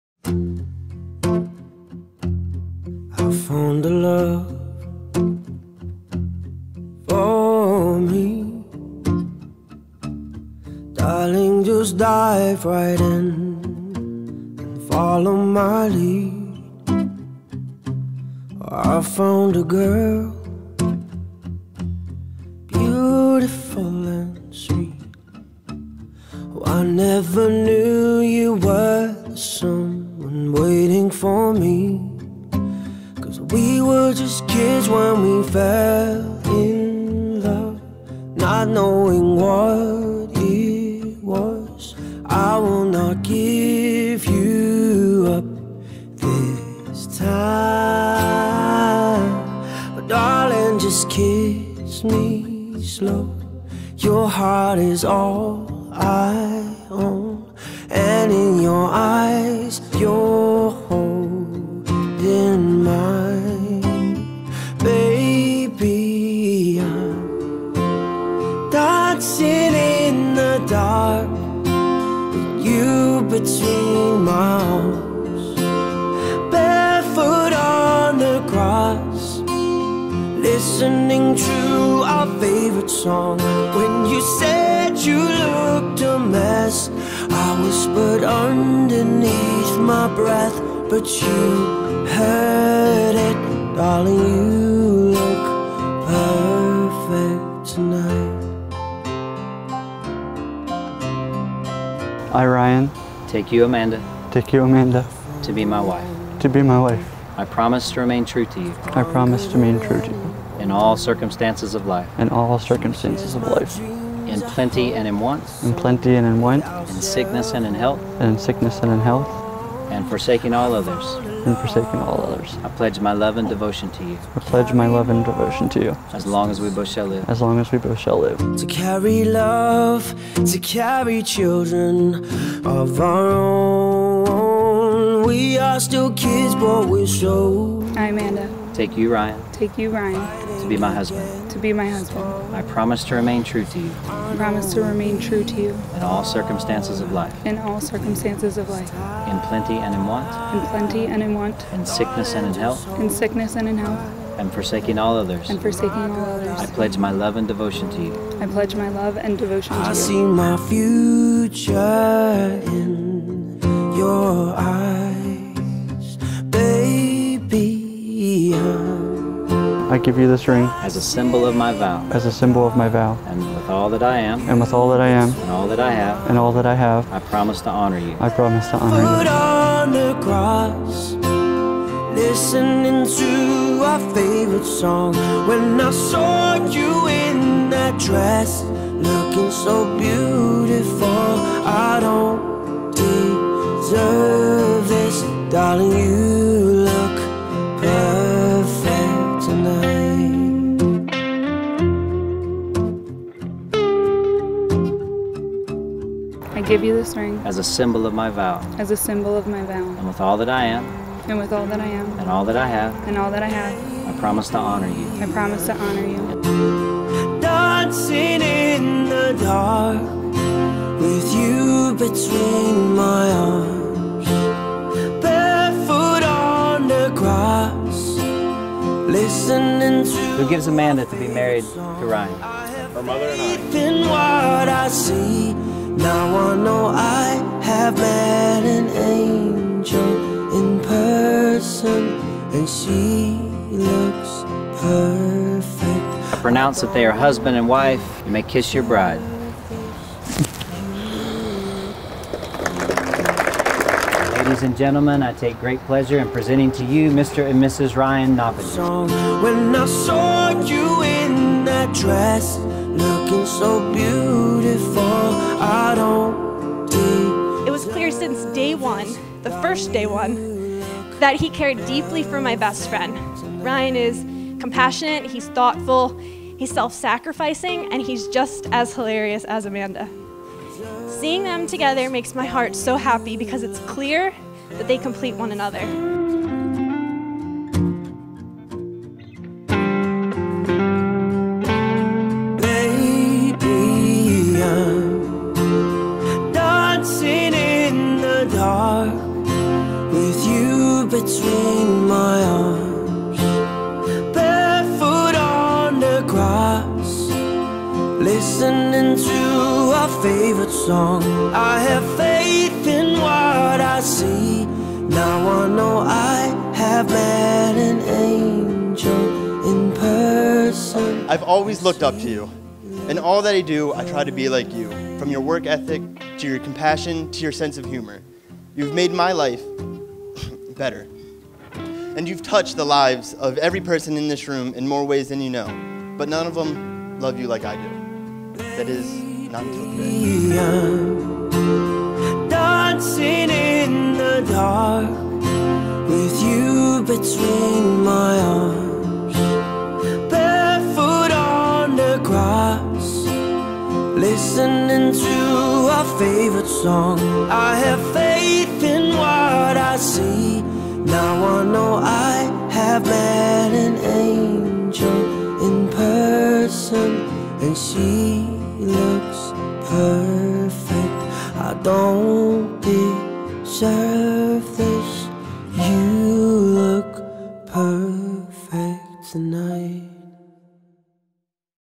Amanda and Ryan - Springfield Manor Winery and Distillery - Thurmont, MD Wedding Video
Amanda and Ryan s wedding video. They were married at Springfield Manor Winery and Distillery in Thurmont, Maryland.